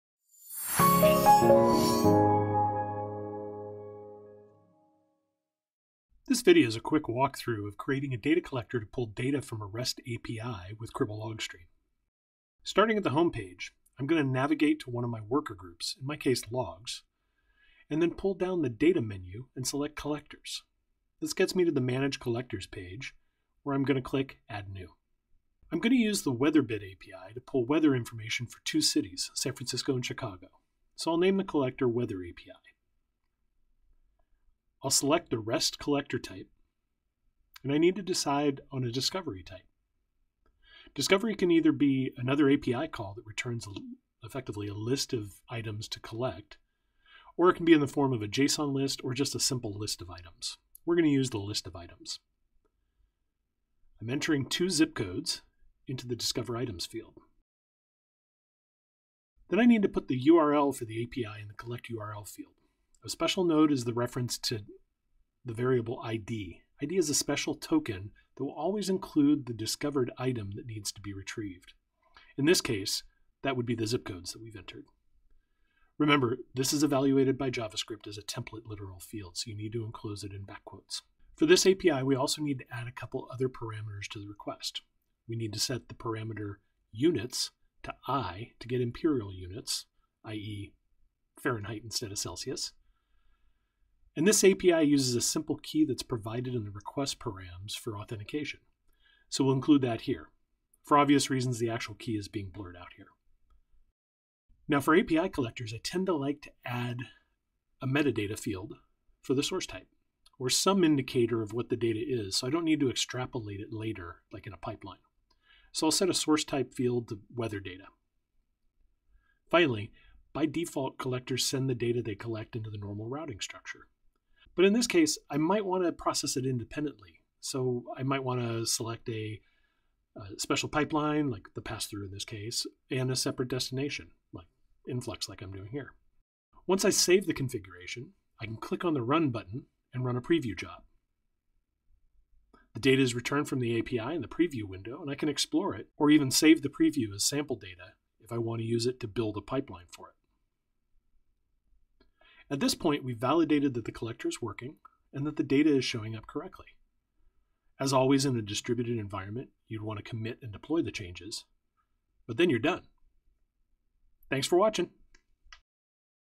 How-To: Configuring a REST API Data Collector
A Walkthrough on creating a data collector to pull data from a REST API.